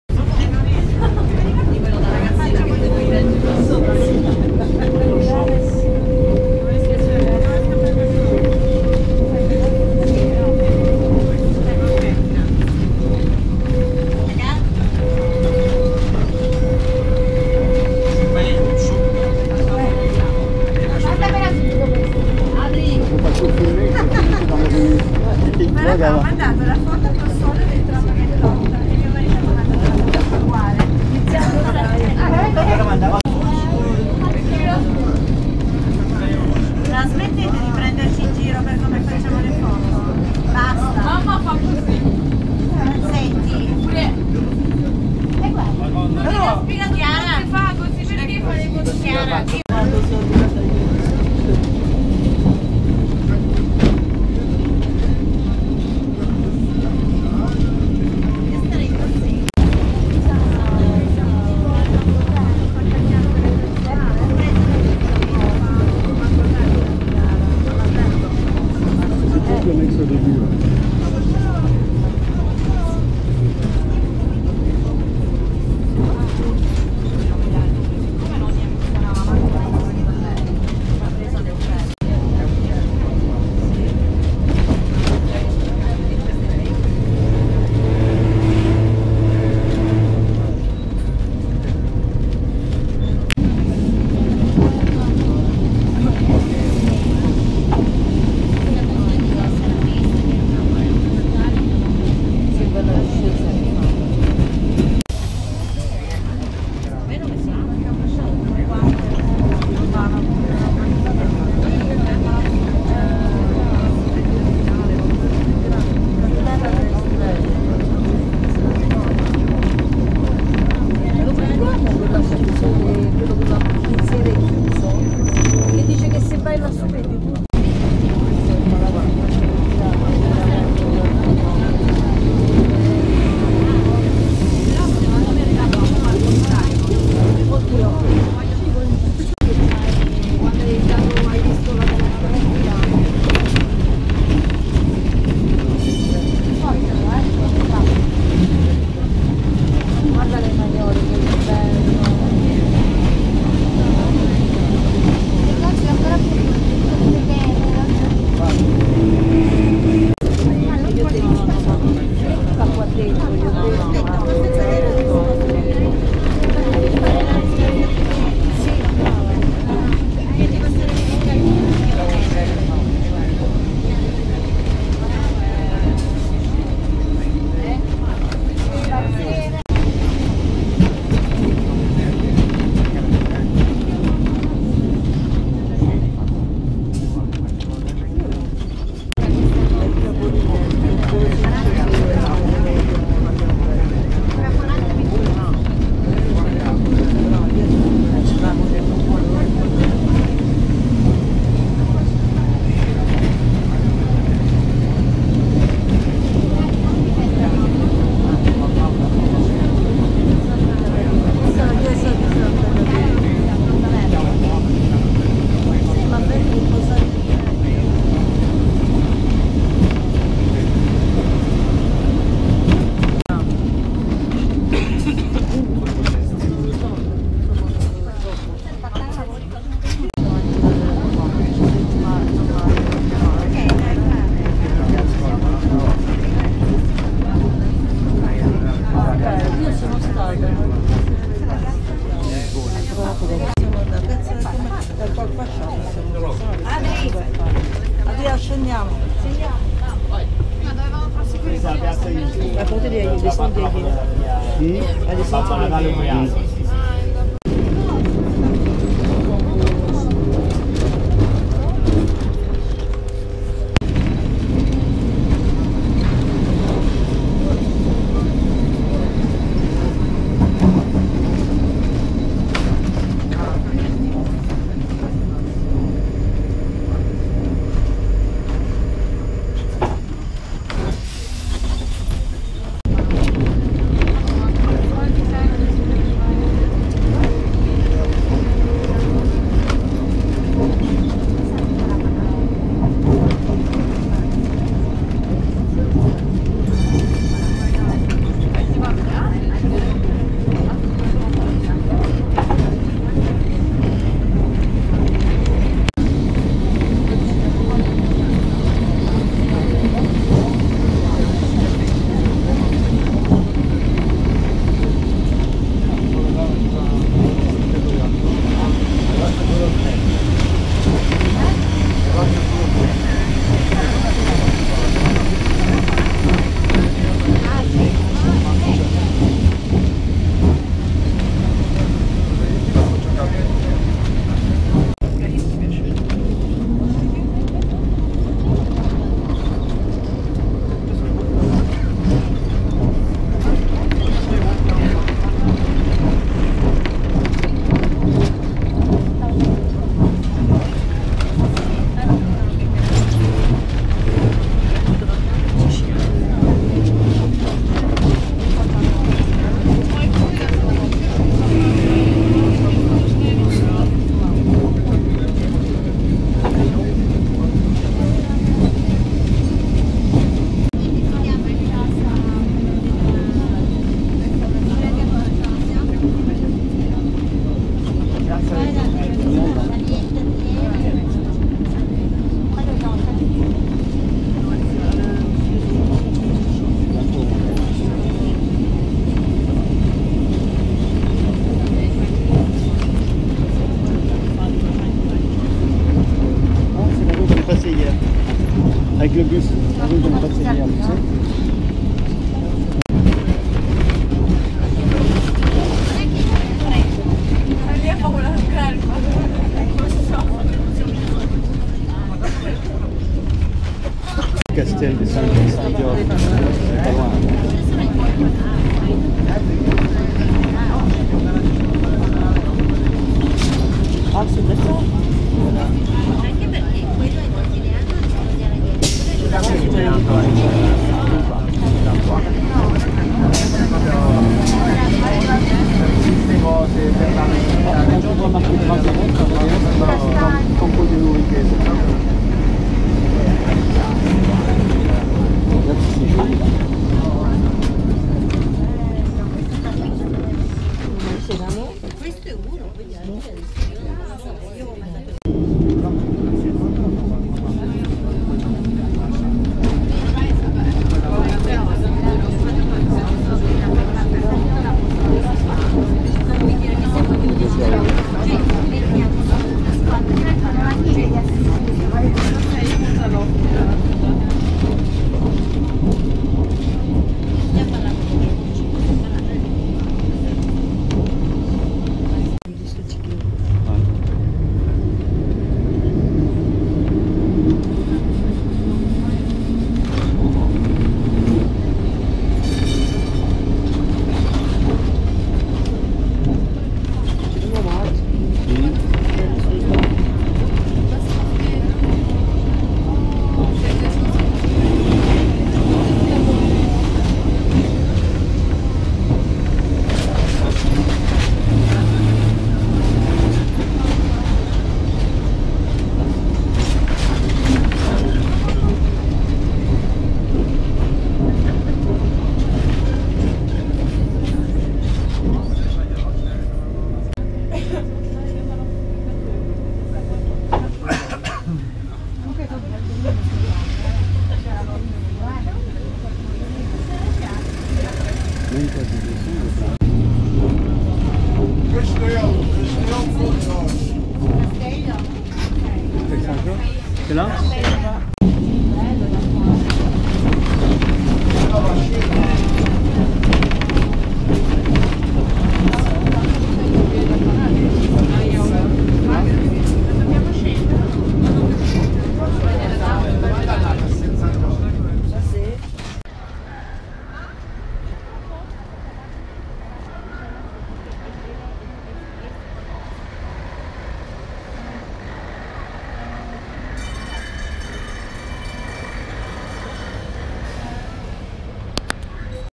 tram 28 Lisbonne - 1
Du terminus Campo Ourique jusqu'à l'arrêt Castel San Jorge avec le fameux tramway de la ligne 28